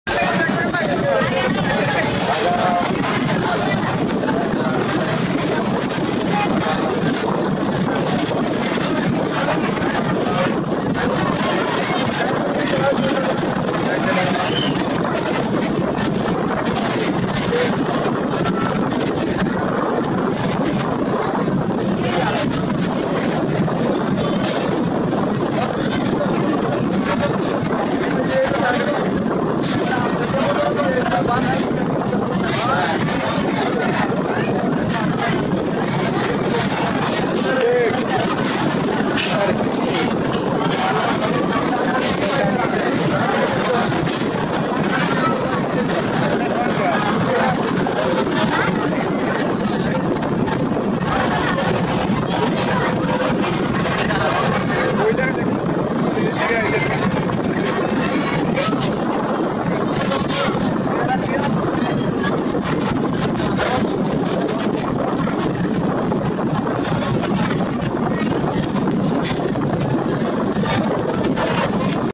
মেঘনায় দোয়েল পাখি-১০ কে ওভারটেক করে ঢাকার পথে ছুটে চলছে গ্রীনলাইন-২|Greenline-2 overtakes Doyelpakhi
মেঘনায় দোয়েল পাখি-১০ কে ওভারটেক করে ঢাকার পথে ছুটে চলছে গ্রীনলাইন-২